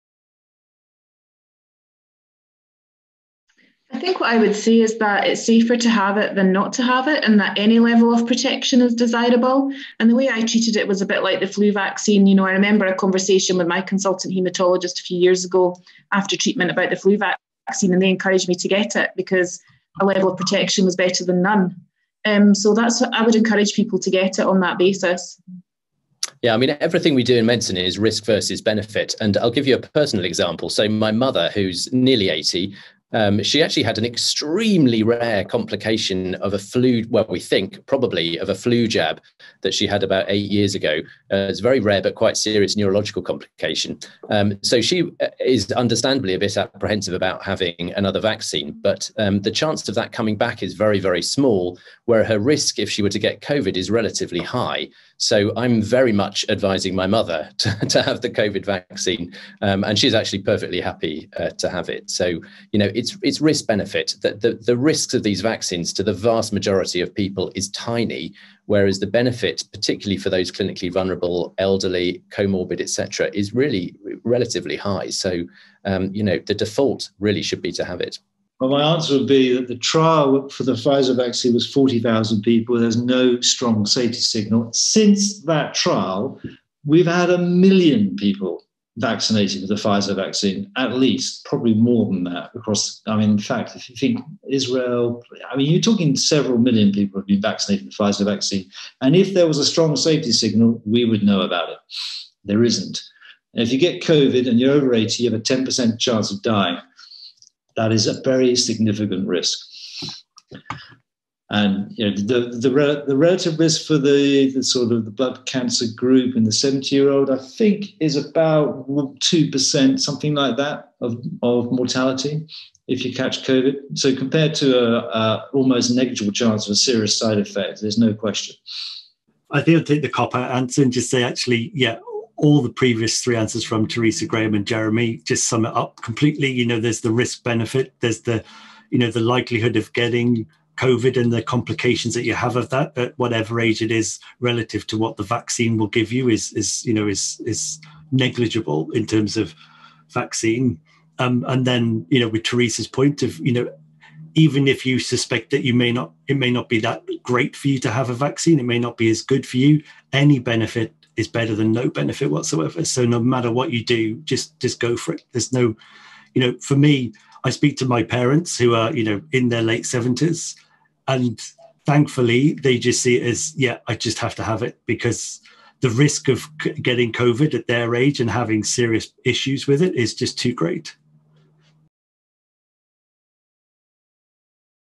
How do I know the vaccine is safe?
Join our experts as they answer your questions on the Covid-19 vaccines.

We are joined by:

Dr Graham Collins, lymphoma consultant and researcher, University of Oxford
Dr Teresa Inkster, consultant microbiologist and affected by blood cancer, NHS Greater Glasgow and Clyde
Professor Edd James, Associate Professor in Cancer Immunology at the University of Southampton
Professor Jeremy Brown, UCLH and member of the JCVI
Rachel Kahn, Research Communications Manager, Blood Cancer UK